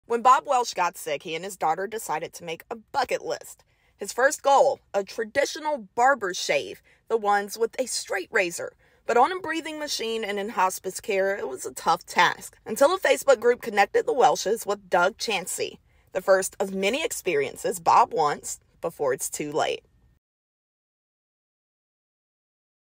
Barber helps man achieve Bucket List goal
Bob Welsh's top bucket list item was an old-school barbershop-style shave. A Facebook post helped him achieve that.